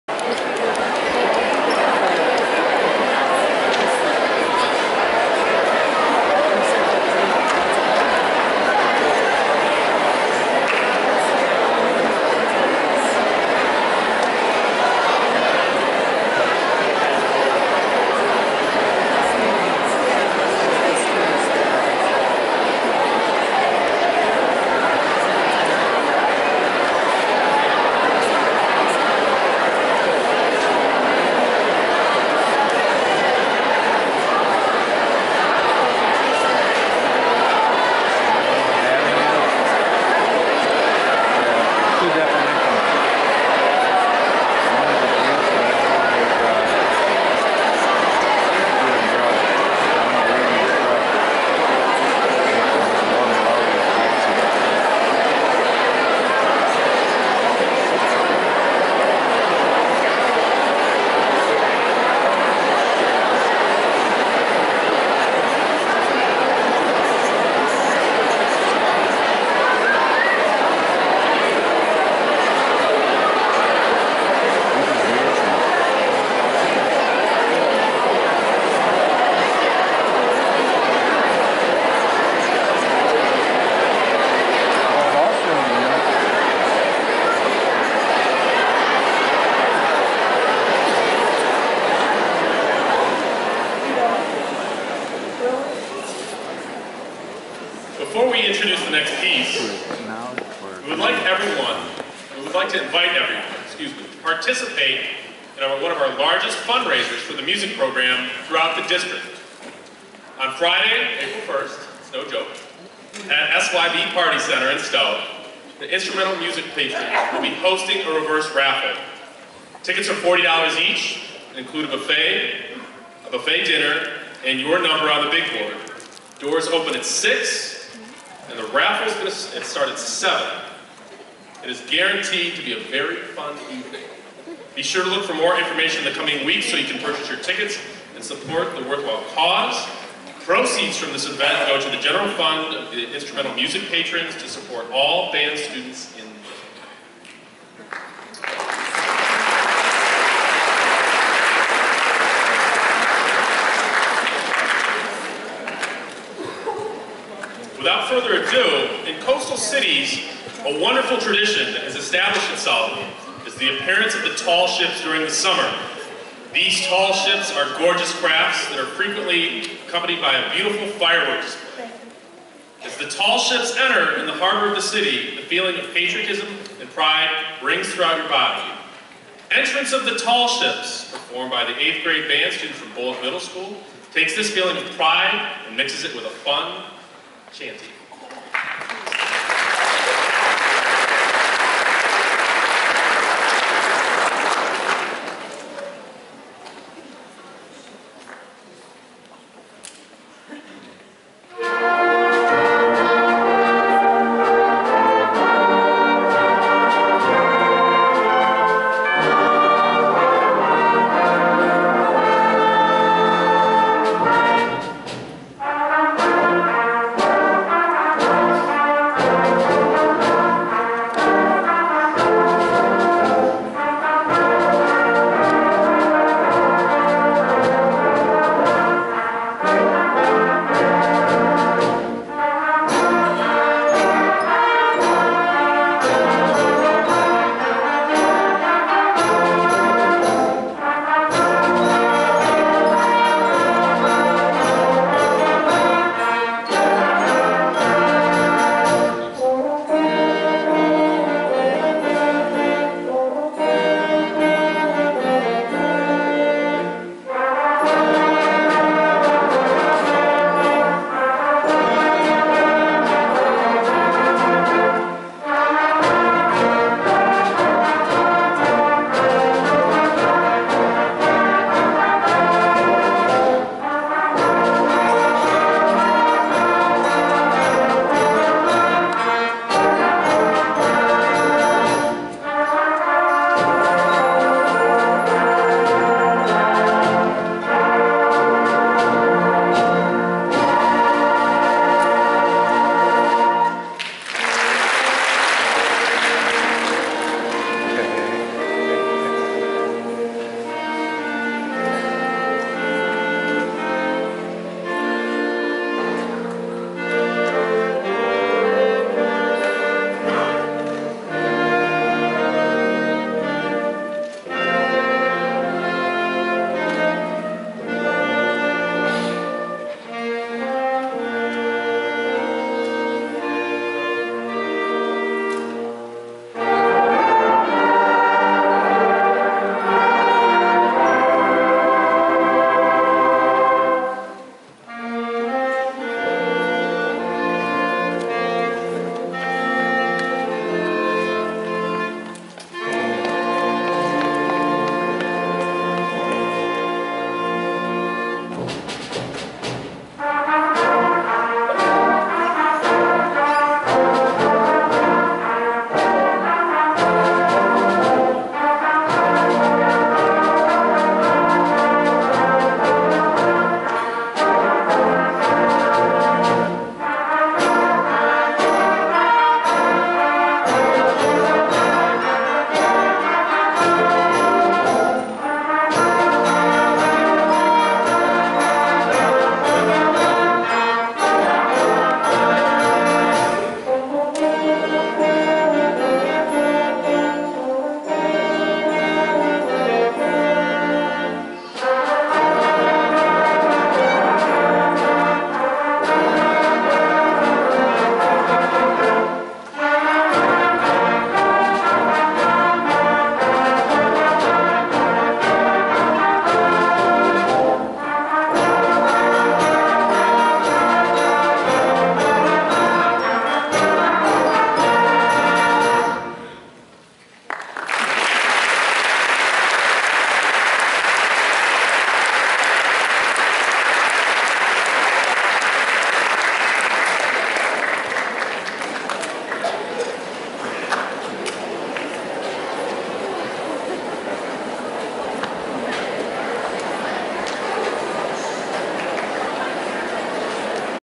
All City Band Cuyahoga Falls - Bolich Middleschool 8th grade band 1/2017
The 8th grade band plays at the All City Band Concert in January of 2017.